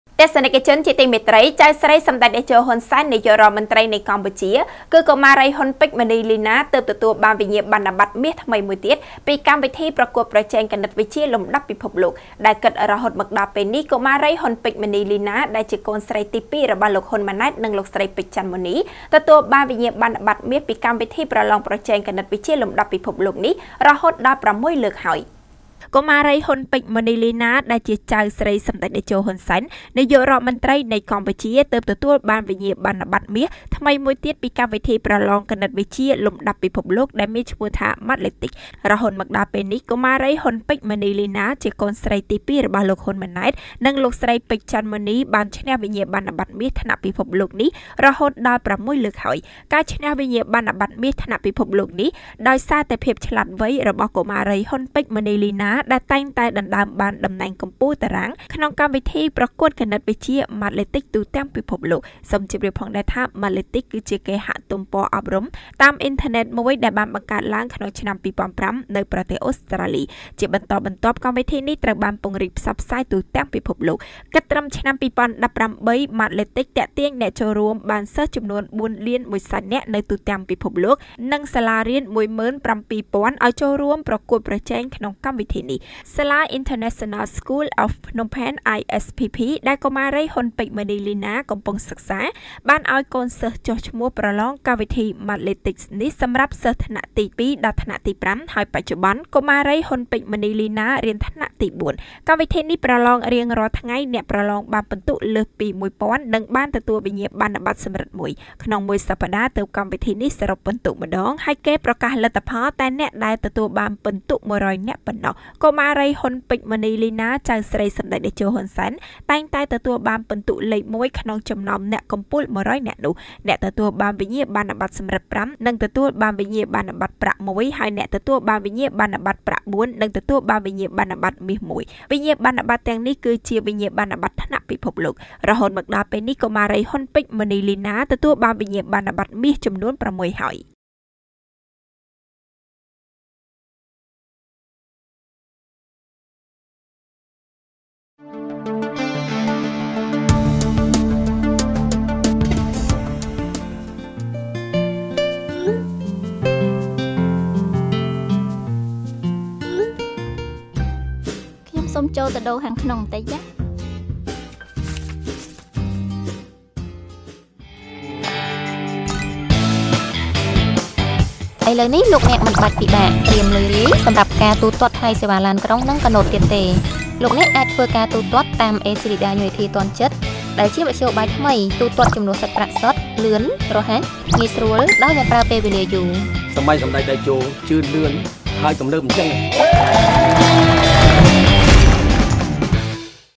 កុមារី ហ៊ុន ពេជ្រមុន្នីលីណា ចៅស្រីសម្តេចតេជោ ហ៊ុន សែន ឈ្នះវិញ្ញាបនបត្រមាស៦លើក.
កុមារី ហ៊ុន ពេជ្រមុន្នីលីណា ចៅស្រីសម្តេចតេជោ ហ៊ុន សែន ឈ្នះវិញ្ញាបនបត្រមាស៦លើក ពីកម្មវិធីប្រឡងគណិតវិទ្យាលំដាប់ពិភពលោក Mathletics
* រាយការណ៍ព័ត៌មានដោយកញ្ញា យ៉ង់ សក្គុណា, ទូរទស្សន៍ព័ត៌មាន Fresh News